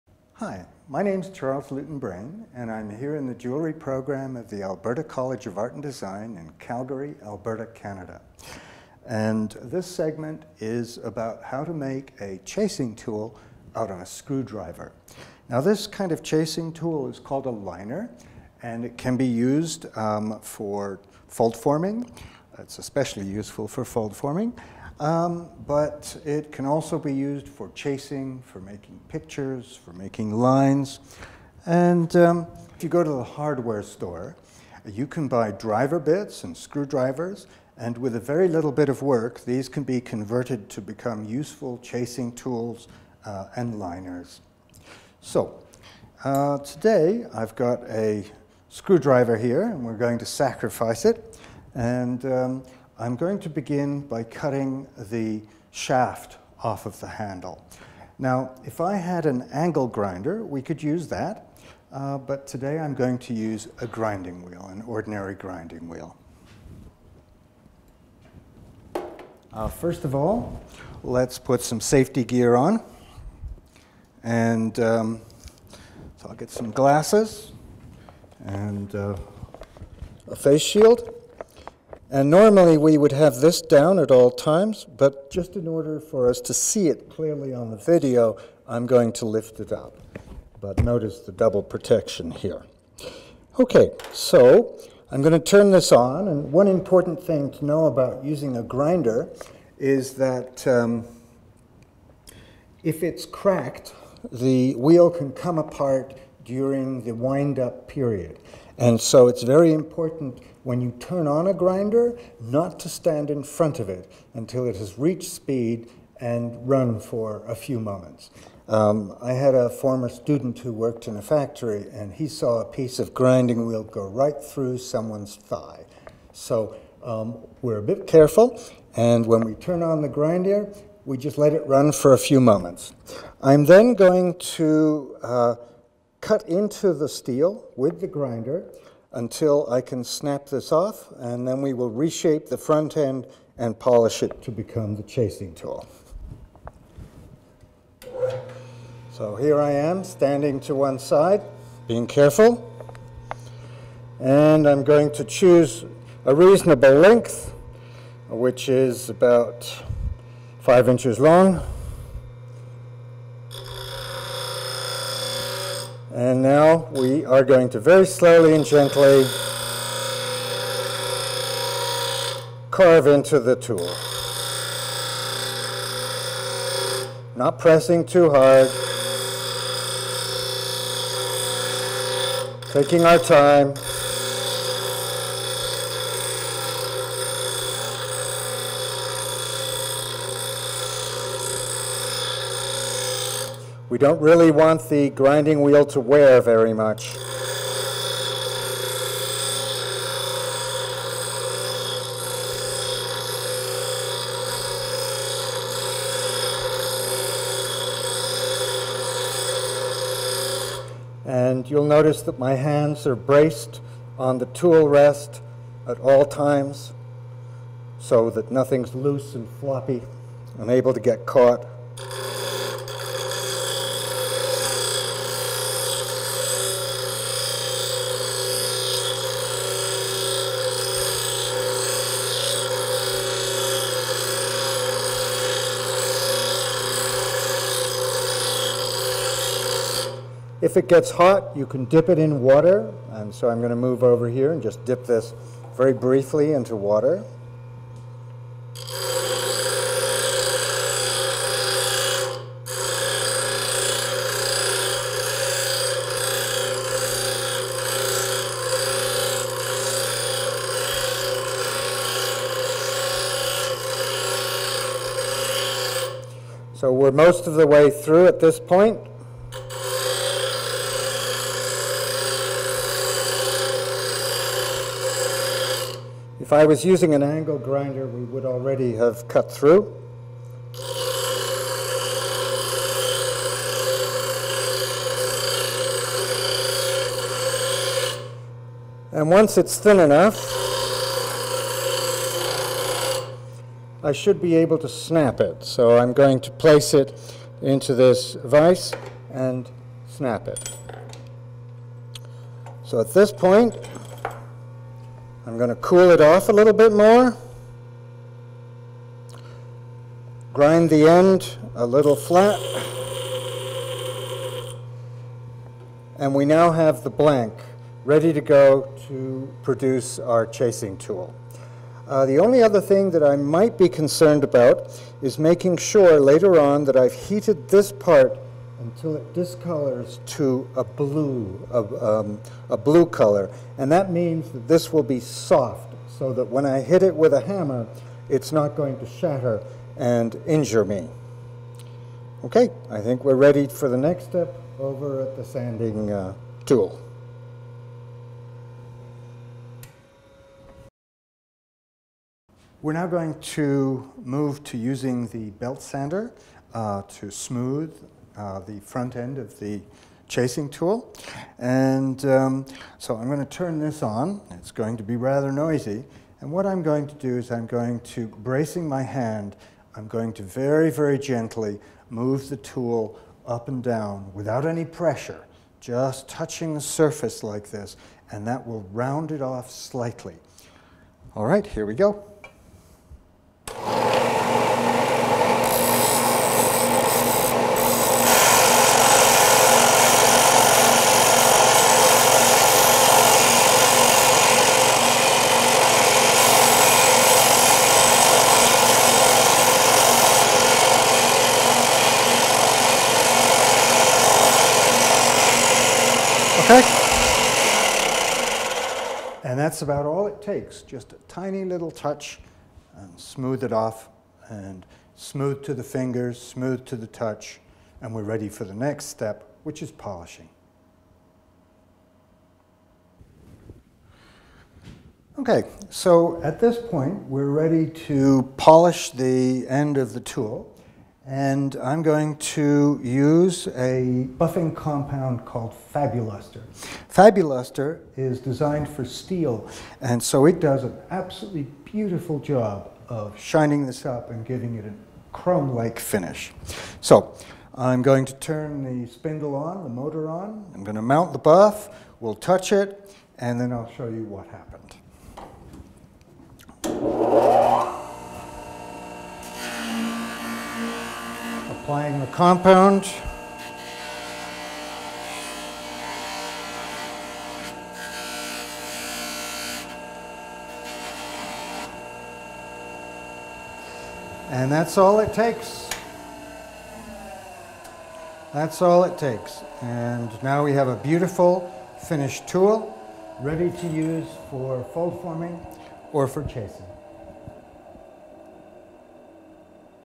Altering a Big Screwdriver to make a Foldforming Liner and Chasing Tool
This is a short video about converting a tool to make a large soft liner for Foldforming and chasing. Here are a few slideshows on making other chasing tools.